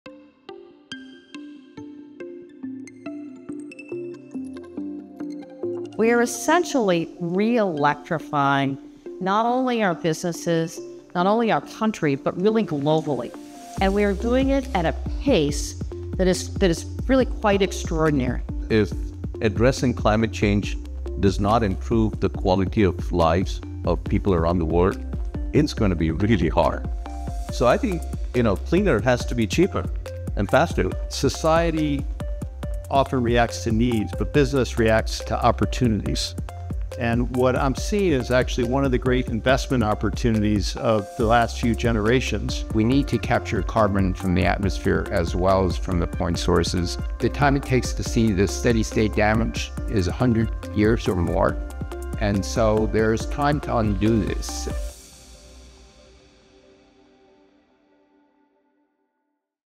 Energy and Decarbonization
Clean energy solutions are both an urgent necessity and a massive opportunity. At the inaugural Business, Government, and Society Forum, leaders explored how accelerating affordable decarbonization can drive investment returns while tackling climate change.